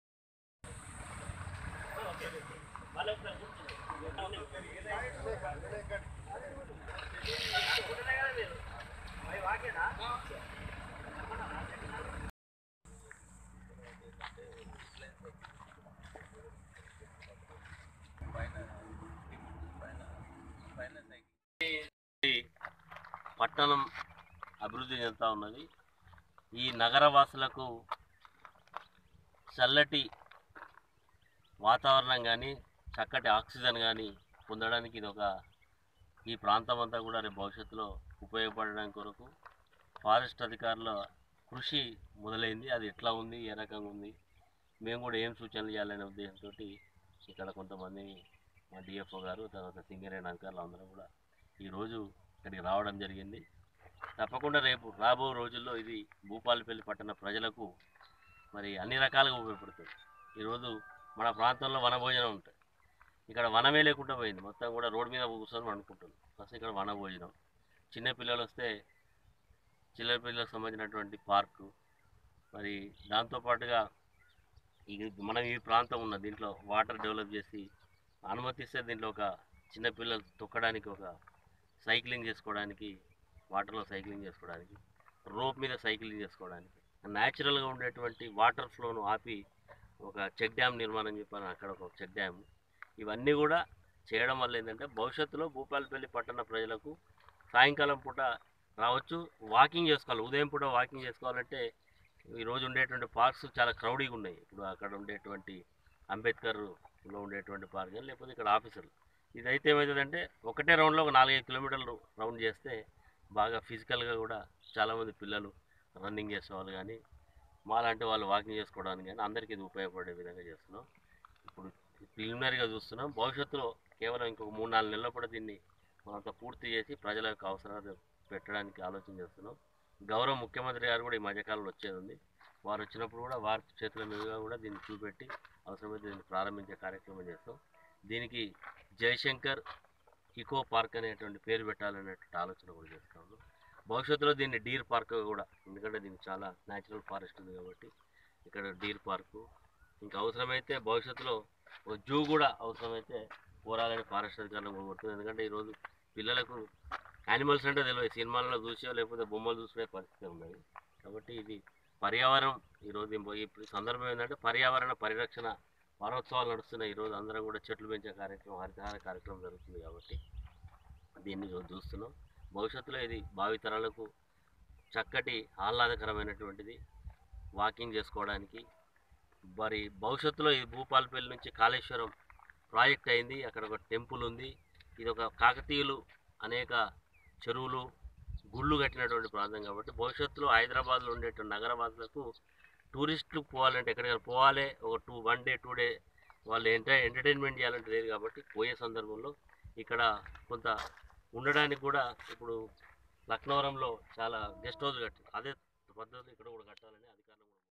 భూపాలపల్లి జిల్లా కేంద్రంలో త్వరలోనే ఎకో పార్క్ ను సీఎం కేసీఆర్ చేతుల మీదుగా ప్రారంభం.|| JSR NEWS
జయశంకర్ భూపాలపల్లి జిల్లా,పట్టణవాసులకు త్వరలోనే ఒక మంచి ఆహ్లాదకరమైన పార్క్ ను ముఖ్యమంత్రికేసీఆర్ చేతులమీదుగా పరంభిస్తాము ఎంఎల్ఏ గండ్ర.